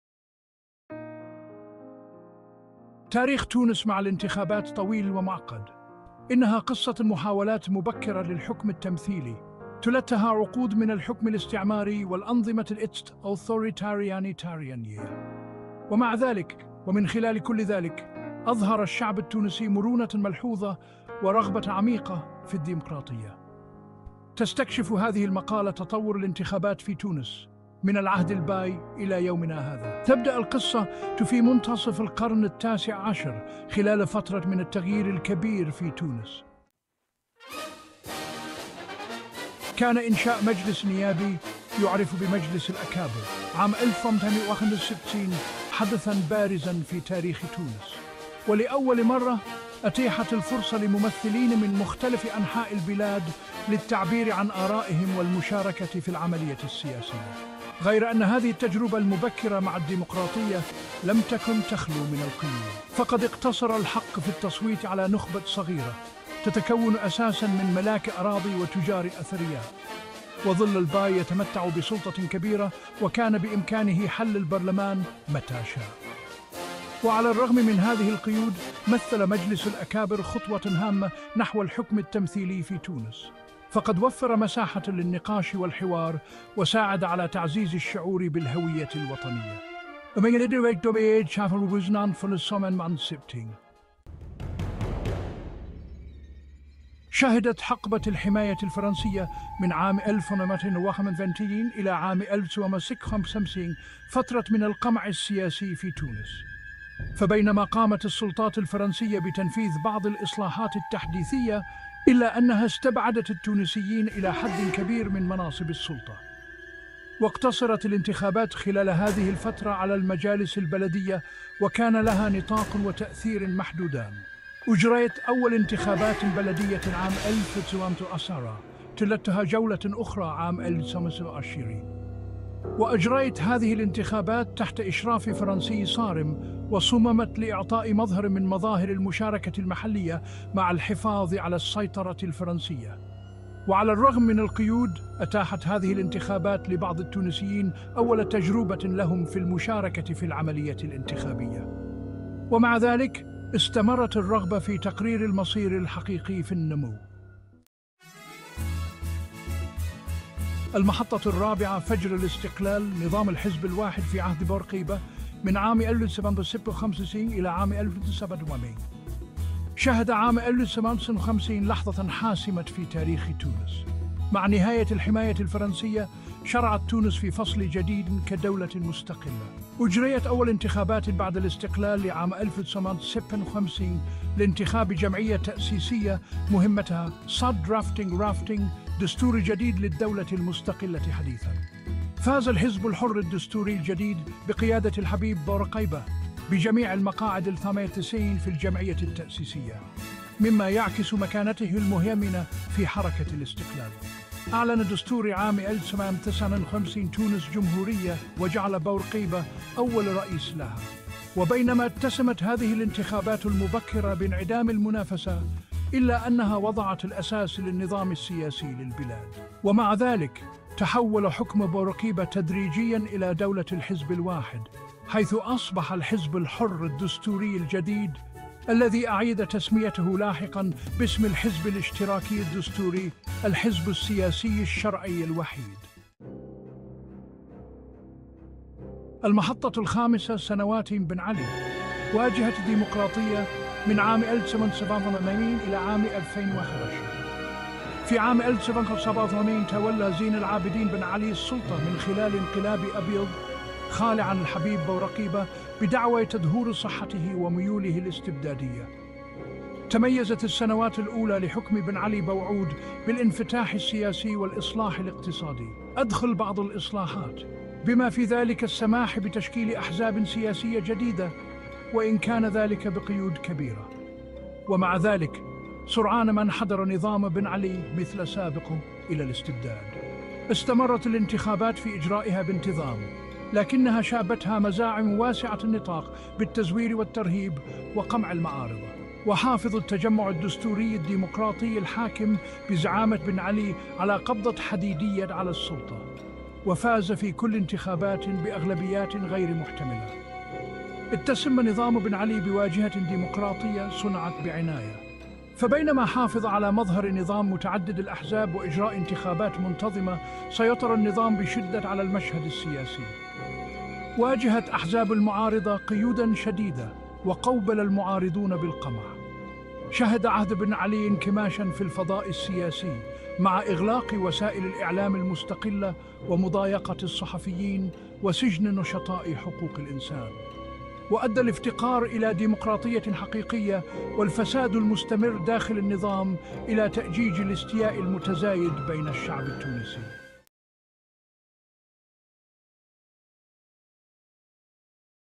تاريخ الانتخابات في تونس: رحلة نحو الديمقراطية
انضم إلينا لاستكشاف التاريخ المثير للانتخابات في تونس، حيث نتتبع الرحلة من عصر البايات إلى الإطار الديمقراطي الحديث الذي تم تأسيسه بعد ثورة 2011. اكتشف اللحظات الرئيسية، بما في ذلك أول لمحة عن التمثيل الانتخابي في عام 1861، والمعاناة خلال الاحتلال الفرنسي، وتأسيس الاستقلال، وتطور الديمقراطية في تونس المعاصرة. يسلط هذا الفيديو الضوء على صمود الشعب التونسي في سعيهم نحو الحرية والتمثيل، ويوفر رؤى قيمة حول أهمية العمليات الانتخابية في تشكيل الهوية السياسية للأمة. لا تنسَ الإعجاب والمشاركة والاشتراك لمزيد من المحتوى المعلوماتي!